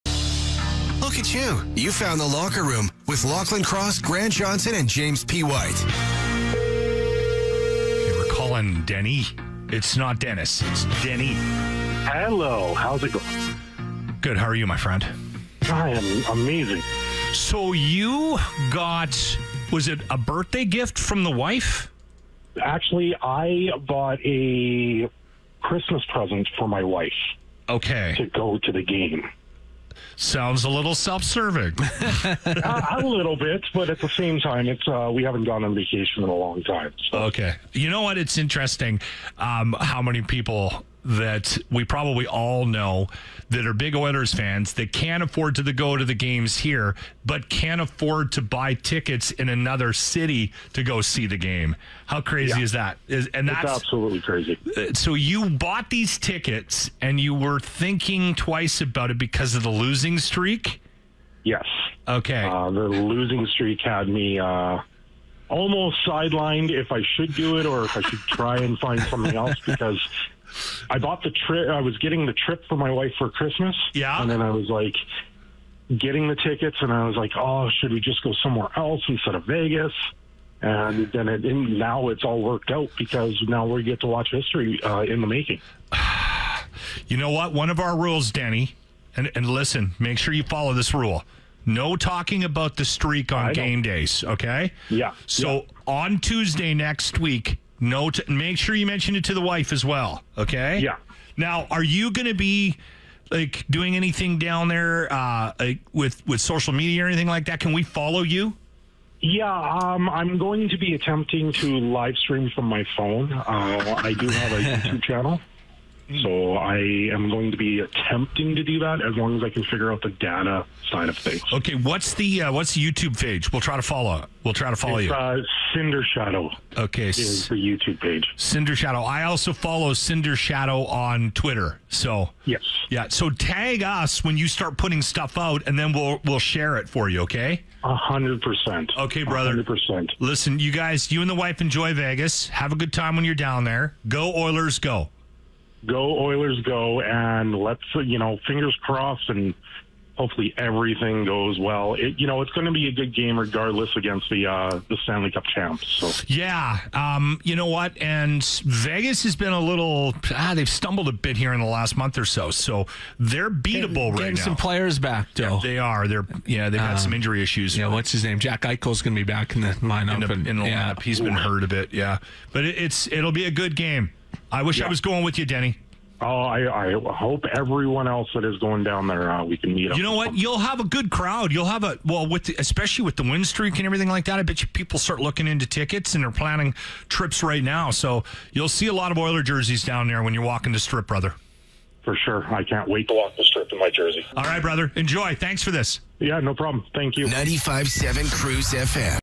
I Was Interviewed For The Vegas Trip! Let's Go OILERS
I was interviewed for a Edmonton Radio Station over the Vegas trip to see the Oilers vs the Knights in Vegas!